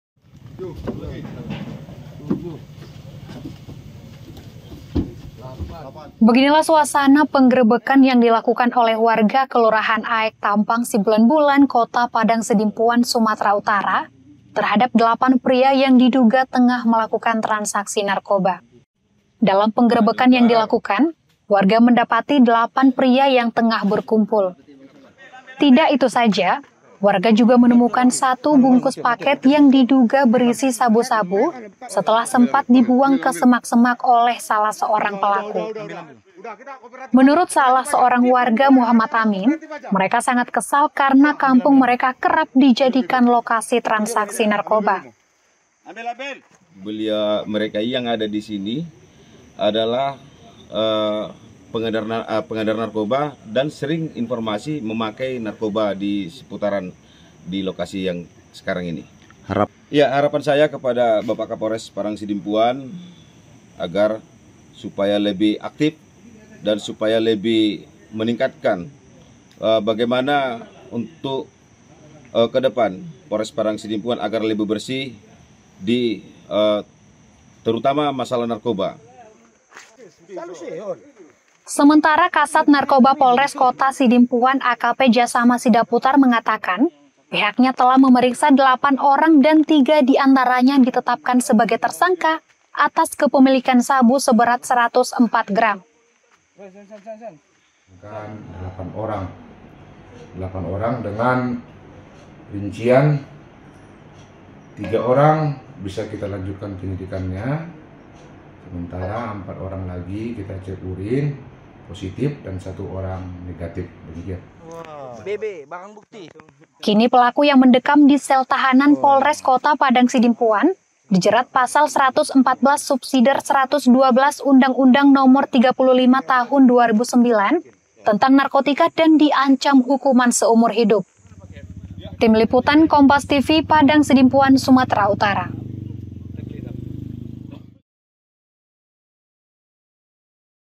Warga Padangsidempuan Tangkap 8 Orang yang Diduga Pelaku Narkoba
PADANGSIDEMPUAN, KOMPAS.TV - Warga Desa Aek Tampang Sibulan-bulan Kota Padangsidempuan menangkap 8 pelaku narkoba.

Saat ditangkap, pelaku didapati sedang bertransaksi narkoba.

Warga juga menemukan satu bungkus paket sabu.

Penangkapan ini dilakukan warga karena kesal kampung mereka dijadikan lokasi transaksi narkoba. (*)